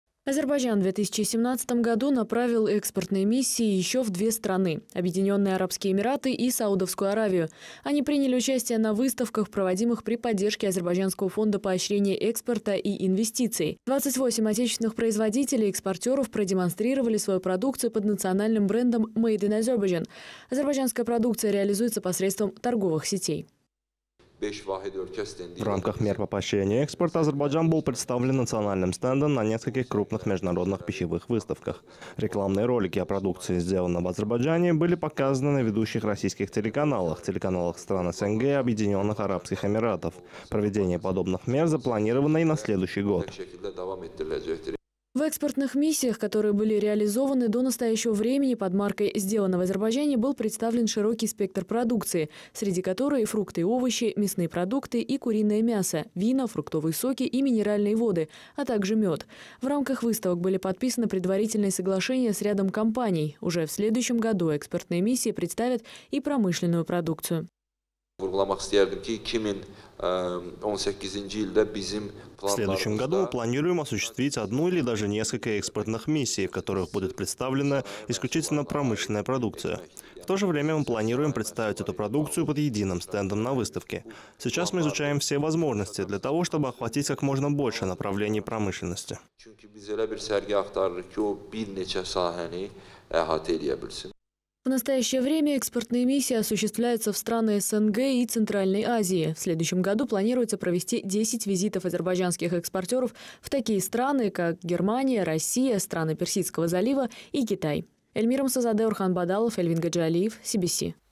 Подведены итоги экспортных миссий Азербайджана в ОАЭ и Саудовскую Аравию
В текущем году Азербайджанский фонд поощрения экспорта и инвестиций (AZPROMO) изучил более 20 потенциальных рынков сбыта местной продукции. Об этом в ходе пресс-конференции сообщил вице-президент AZPROMO Юсиф Абдуллаев. За прошлый год в зарубежные страны было отправлено 10 экспортных миссий. Их результатом стало 30 заключенных экспортных соглашений, более 10 из которых находятся в стадии реализации.
Азербайджан в 2017 году направил экспортные миссии еще в две страны – Объединенные Арабские Эмираты и Саудовскую Аравию. Они приняли участие в выставках, проводимых при поддержке Азербайджанского фонда поощрения экспорта и инвестиций. 28 отечественных производителей и экспортеров продемонстрировали свою продукцию под национальным брендом Made in Azerbaijan. Азербайджанская продукция реализуется посредством торговых сетей.
"В экспортных миссиях, которые были реализованы до настоящего времени, под брендом Made in Azerbaijan был представлен широкий спектр продукции, среди которой фрукты и овощи, мясные продукты и куриное мясо, вина, фруктовые соки и минеральные воды, а также мед. В рамках выставок были подписаны предварительные соглашения с рядом компаний. Уже в следующем году экспортные миссии представят и промышленную продукцию, – сказал Ю.Абдуллаев. – В 2018 году мы планируем осуществить одну или даже несколько экспортных миссий, в ходе которых будет представлена исключительно промышленная продукция. В то же время мы планируем представить эту продукцию под единым стендом на выставке. Сейчас мы изучаем все возможности для того, чтобы охватить как можно больше направлений промышленности".
В настоящее время экспортные миссии осуществляются в страны СНГ и Центральной Азии. В следующем году планируется провести 10 визитов азербайджанских экспортеров в такие страны, как Германия, Россия, государства Персидского залива и Китай.